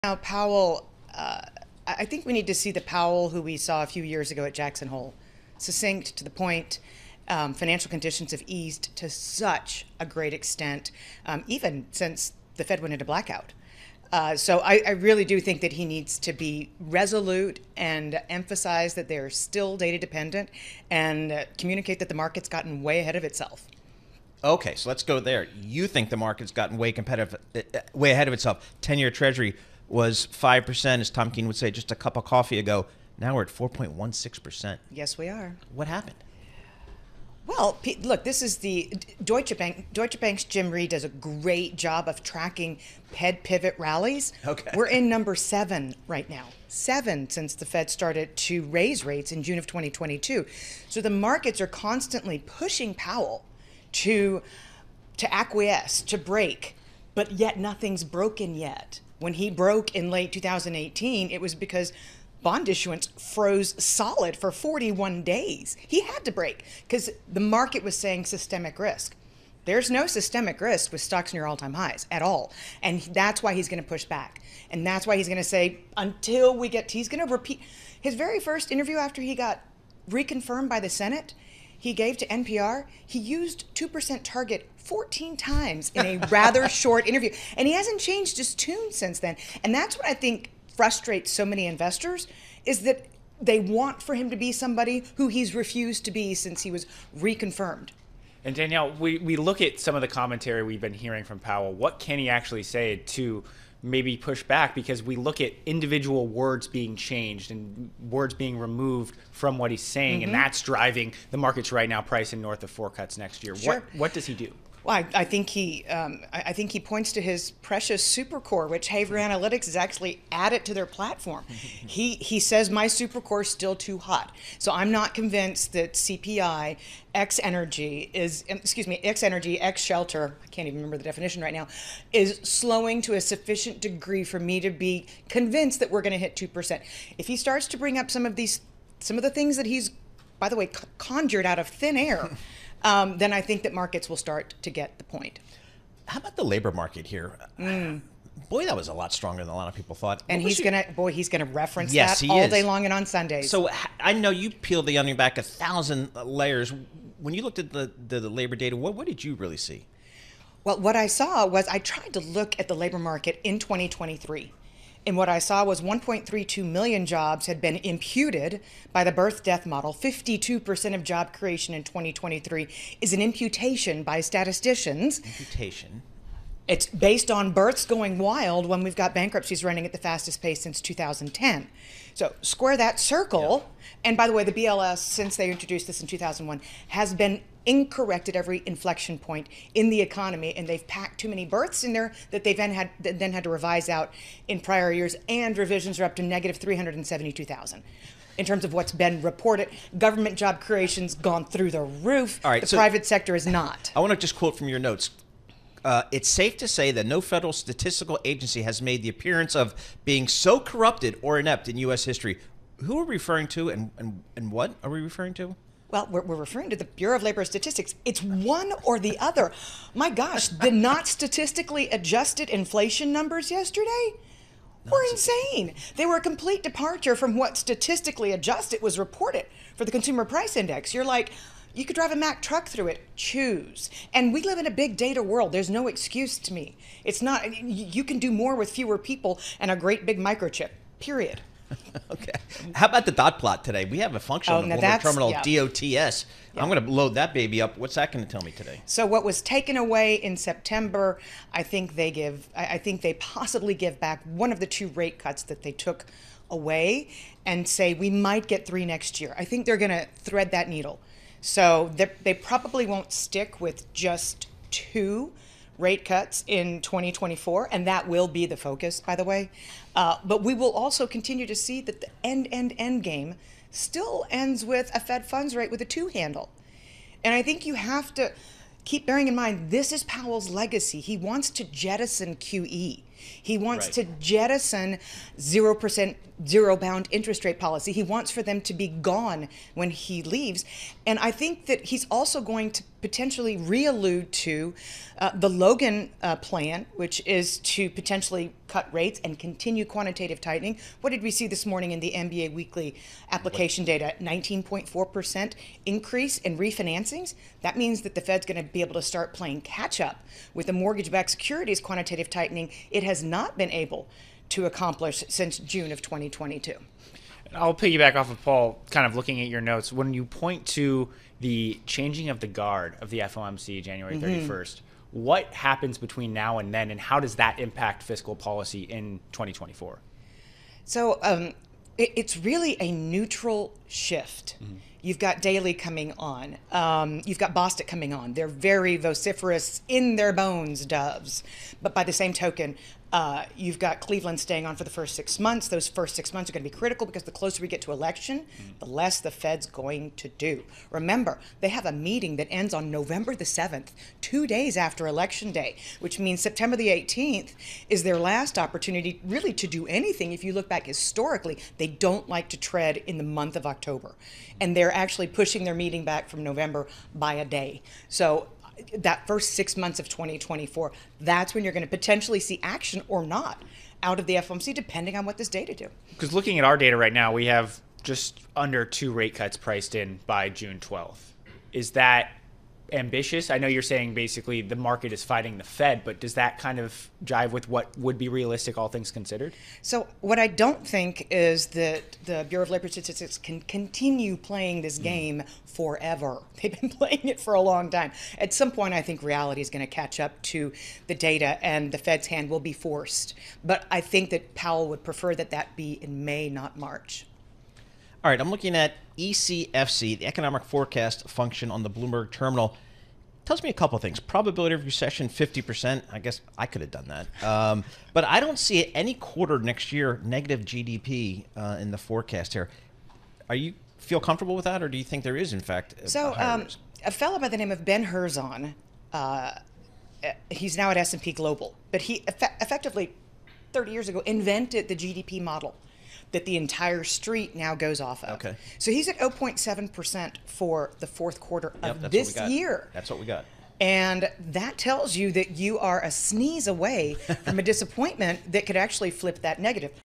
Danielle DiMartino Booth: The market has gotten ahead of itself
Danielle DiMartino Booth, CEO and Chief Strategist at QI Research, joins Bloomberg Radio to discuss the FOMC decision